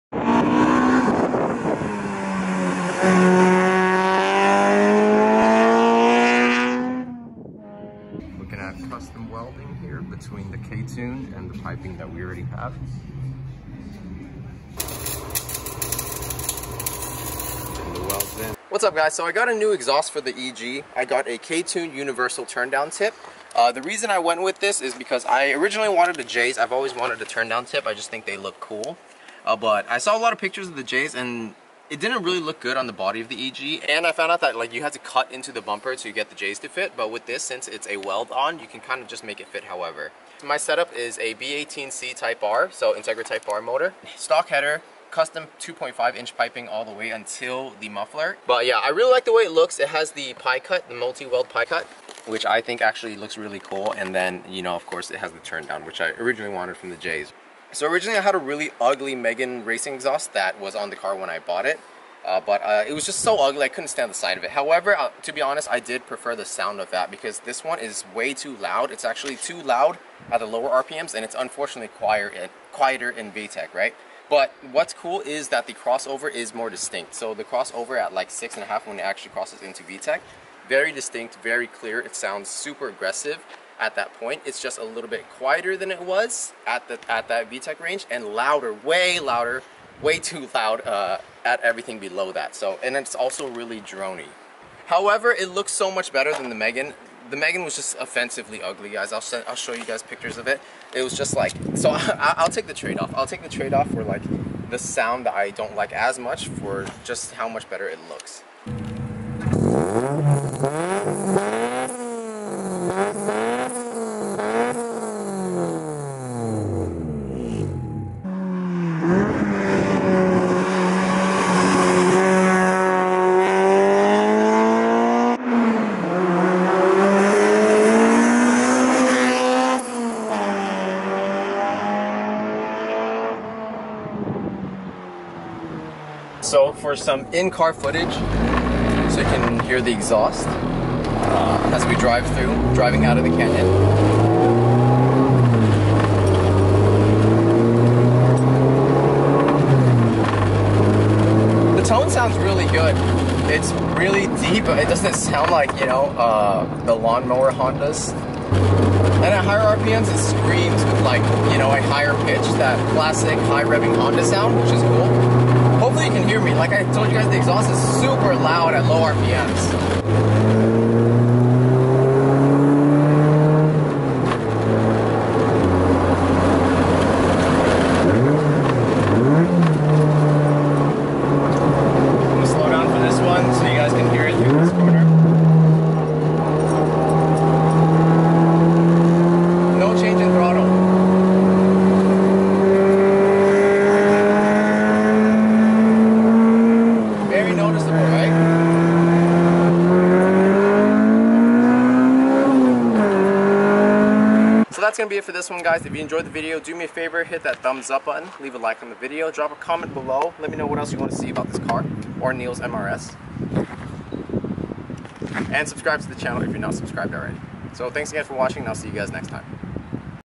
K-Tuned Exhuast: B18C EG Civic (& Integra)
B18C Type R
Stock Header
2.5 inch piping
K tuned Universal Turndown muffler